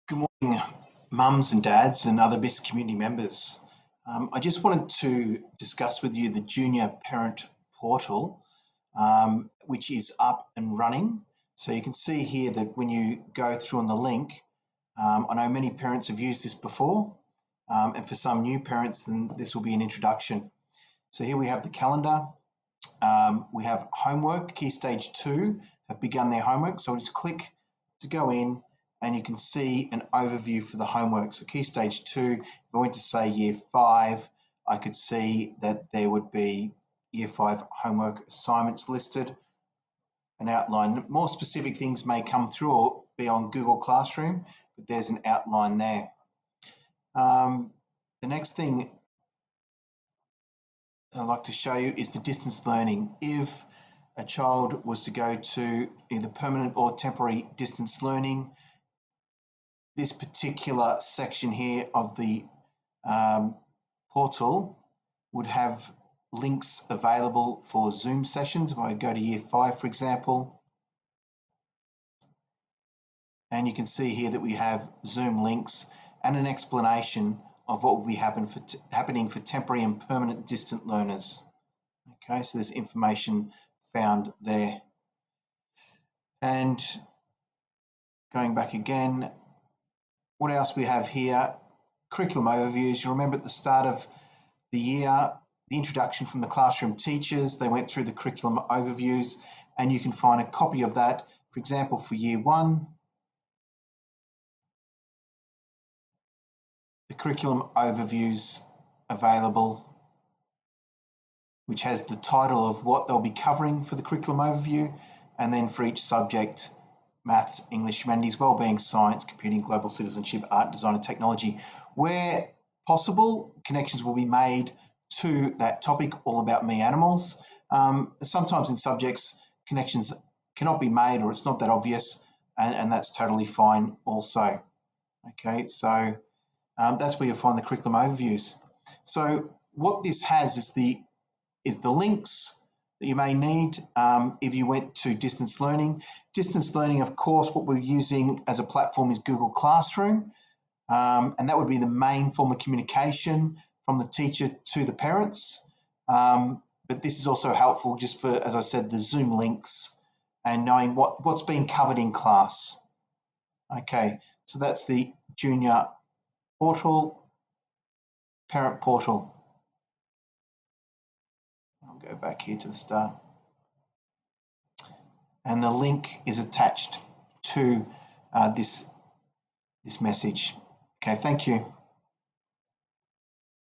Junior Parent Portal
This video gives a quick tour of the Junior School Parent Portal.